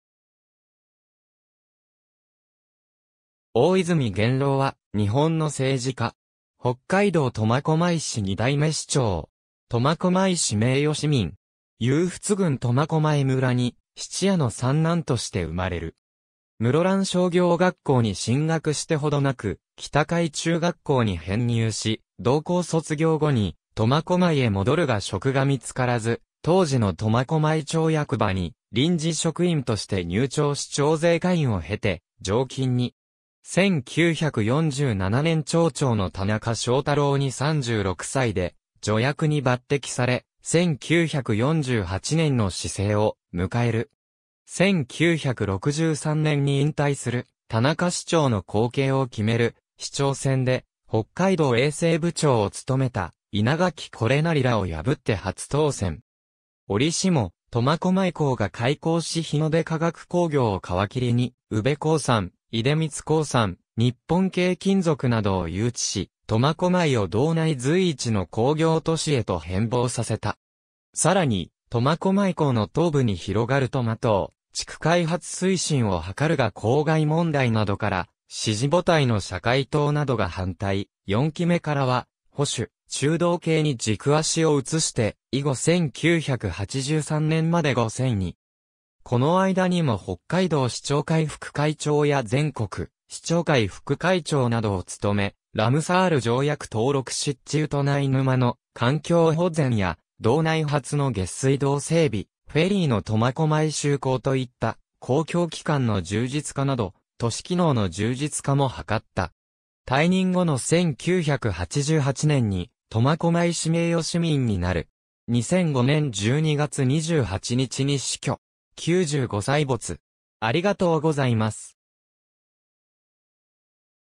大泉源郎
大泉 源郎（おおいずみ げんろう、1910年12月20日 - 2005年12月28日）は、日本の政治家。
北海道苫小牧市2代目市長（1963年5月～1983年4月）。
苫小牧市名誉市民。
勇払郡苫小牧村（現・苫小牧市）に質屋の三男として生まれる。
室蘭商業学校に進学して程無く北海中学校（旧制）に編入し（同級生として坊屋三郎がいる）、同校卒業後に苫小牧へ戻るが職が見つからず、当時の苫小牧町役場に臨時職員として入庁し徴税課員を経て常勤に。
1947年町長の田中正太郎に36歳で助役に抜擢され、1948年の市制を迎える。
1963年に引退する田中市長の後継を決める市長選で、北海道衛生部長を務めた稲垣是成らを破って初当選。
折しも苫小牧港が開港し日之出化学工業を皮切りに宇部興産・出光興産・日本軽金属などを誘致し、苫小牧を道内随一の工業都市へと変貌させた。
更に苫小牧港の東部に広がる苫東地区開発推進を図るが公害問題などから支持母体の社会党などが反対、4期目からは保守・中道系に軸足を移して以後1983年まで5選に。
この間にも北海道市長会副会長や全国市長会副会長などを務め、ラムサール条約登録湿地ウトナイ沼の環境保全や、道内初の下水道整備、フェリーの苫小牧就航といった公共機関の充実化など都市機能の充実化も図った。
退任後の1988年に苫小牧市名誉市民になる。
2005年12月28日に死去。
95歳没。